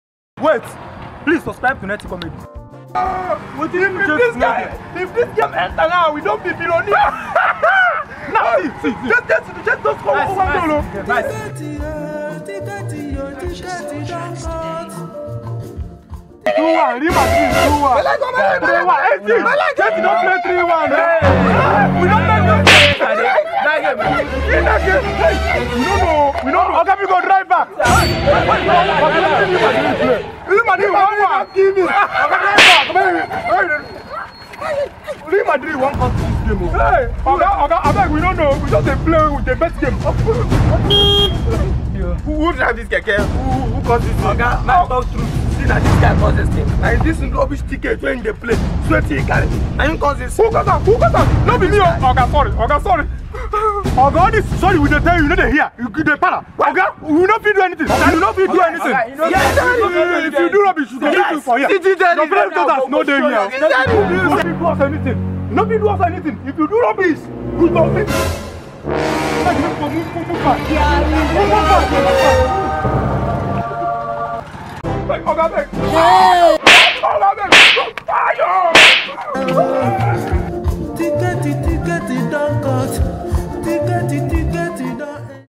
Ticket Bet man on this 😁😁 (official nety wakno
🤣🤣bet man wakno got into trouble🤣🤣
With his friends badoo and netywork
Ticket palava😀🤣😁(nety comedy)bet man wakno,badoo,netywork
Official nety wakno